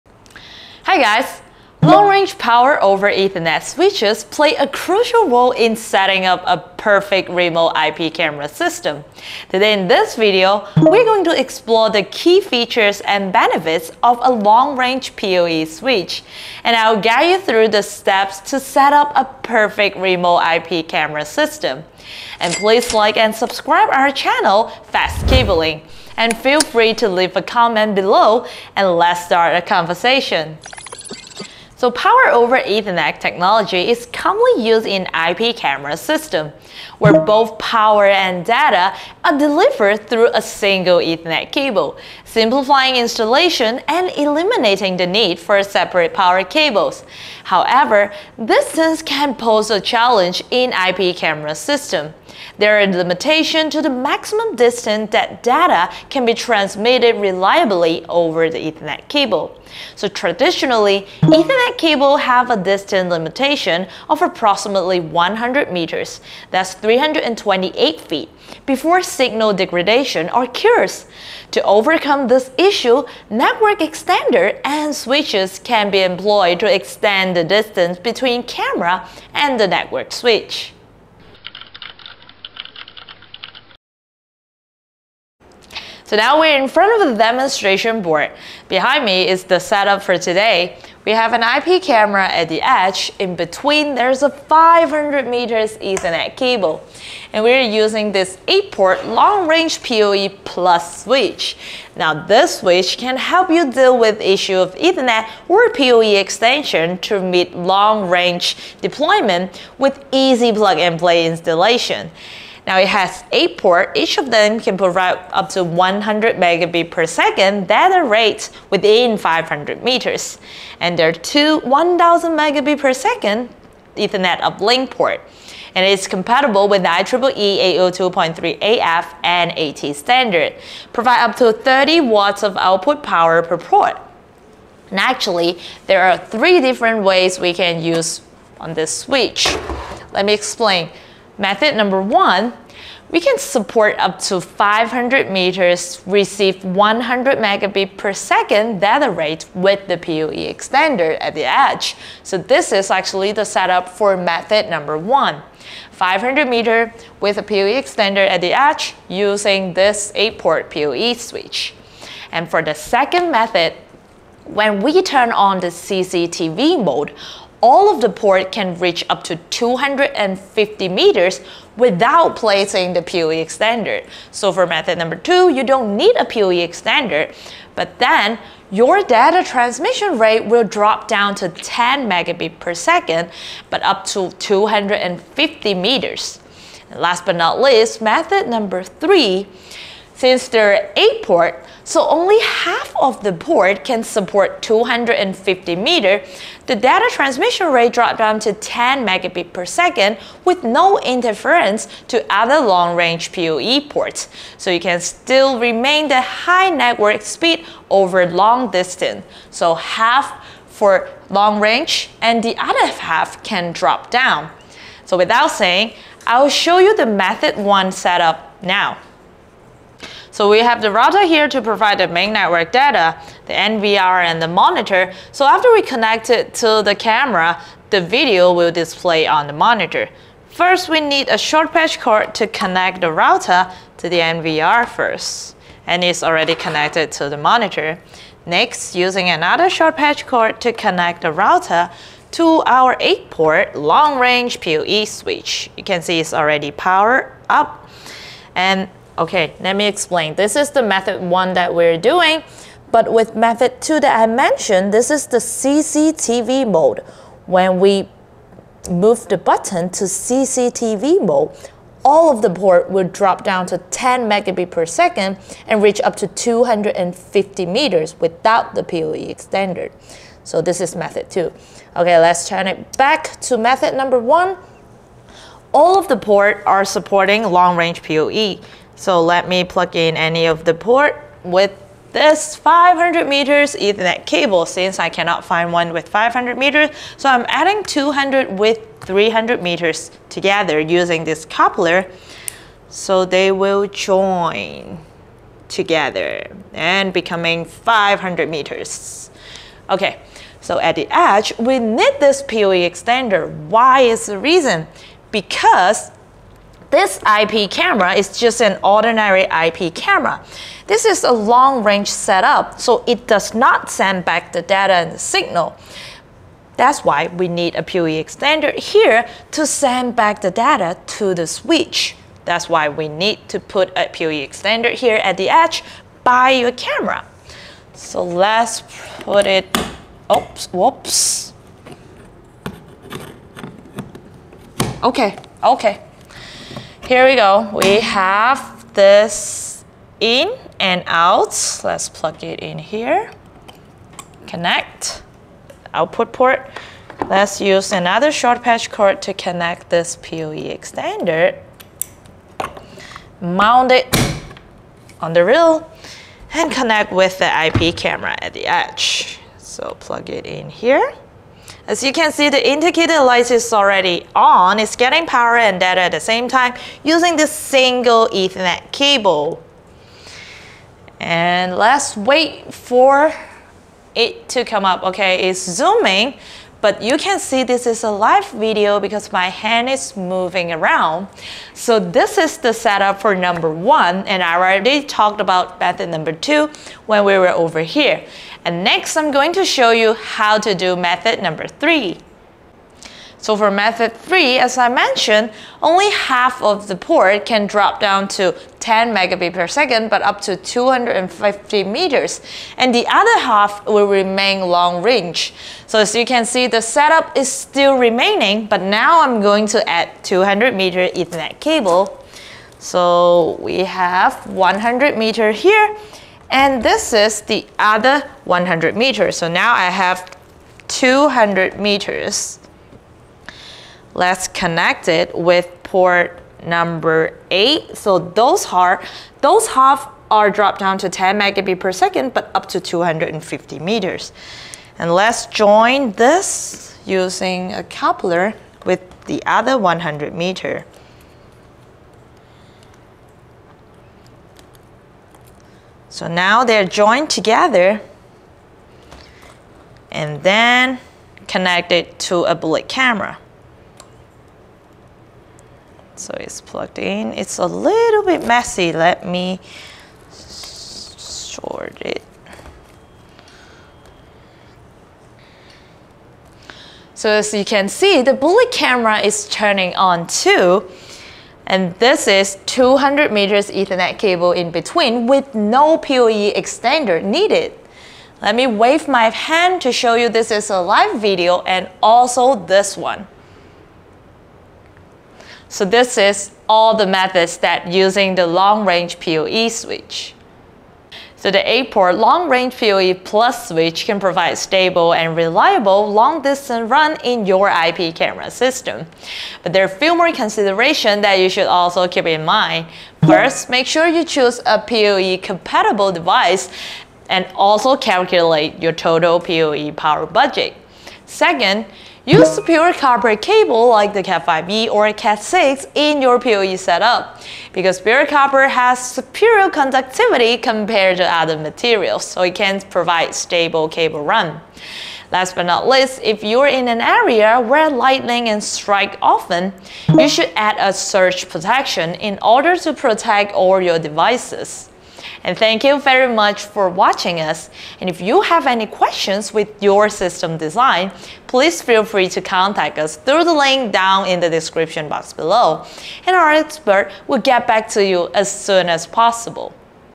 Long Range PoE Switches for the Ultimate Remote IP Camera Setup
Unlock the potential of remote IP camera setups with long-range PoE switches. In this video, we explore how long-range Power over Ethernet (PoE) switches can provide seamless connectivity and power supply to IP cameras installed in remote locations.

Intro
IP Camera System Network & Power
Introduction to 8 Port Long Range PoE Switch
How to Reach Long Range Setup?